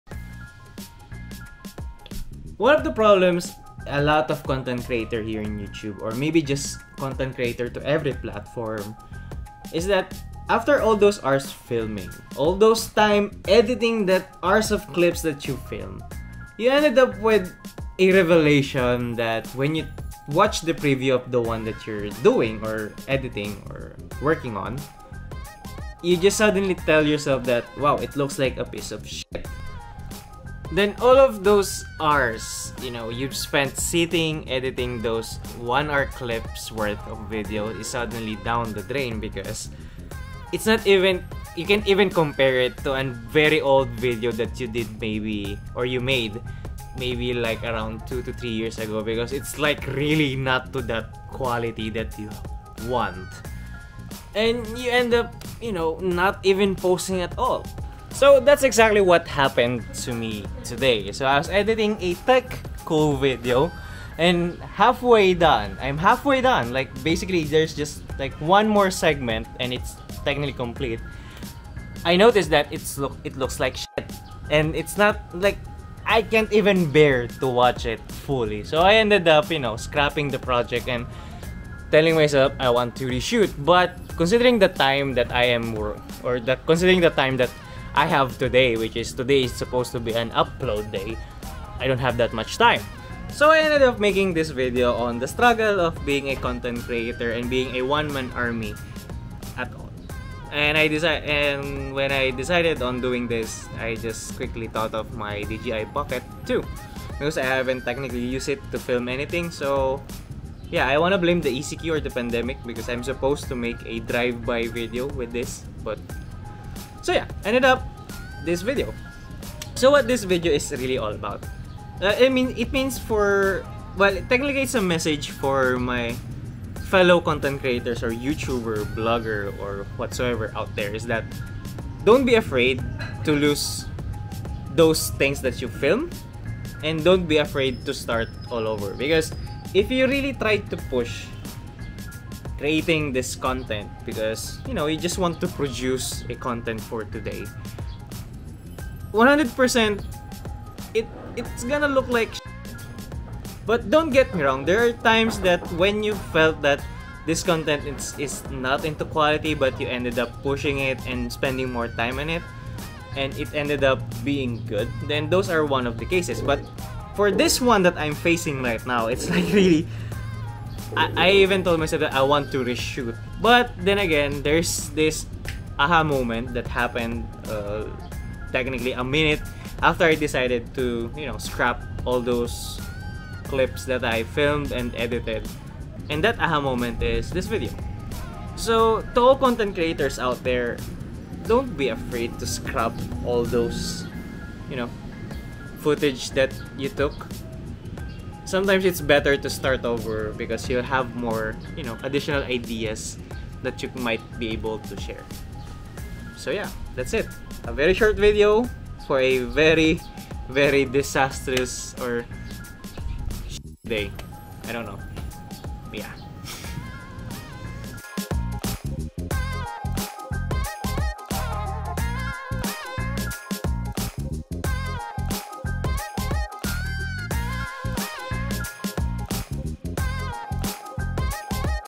Don't Be Afraid To Start Over | DJI Pocket 2 | Vlog #7?
Was suppose to upload a tech video today but... sh** happens.

A short message to my fellow content creators out there